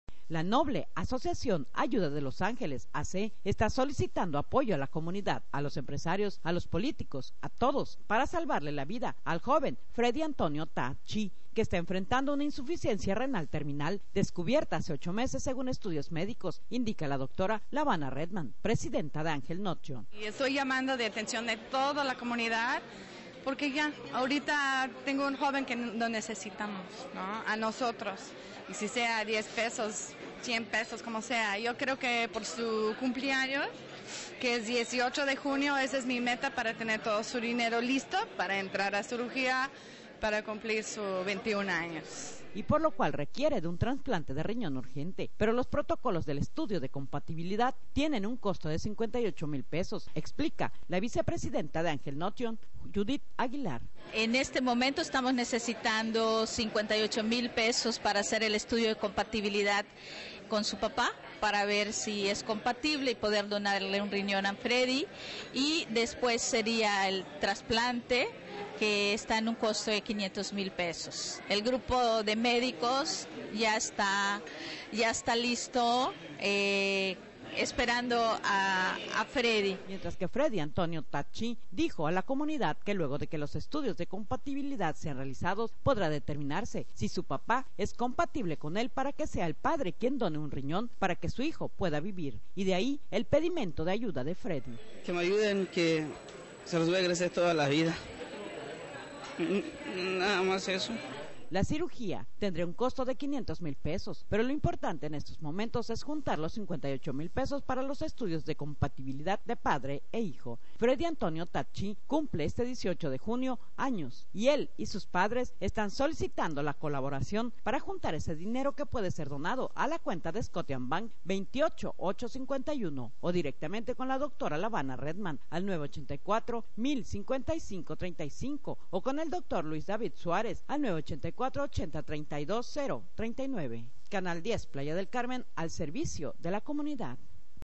¡Ayúdame para salvar mi vida!
El jovencito de 20 años, Freddy Antonio Tah Chi, diagnosticado con una insuficiencia renal terminal, descubierta hace ocho meses, solicita de la comunidad, de los políticos, de los funcionarios y del gobierno, ayuda, para recaudar 58 mil pesos que valen los estudios de compatibilidad que le permitirían a su padre donarle un riñón y así prolongar su vida. La A.C. Ayuda de los Angeles que dirige la doctora Lavanna Redman, convocó a la comunidad médica para que se una en esta cruzada y logren salvarle la vida a Freddy.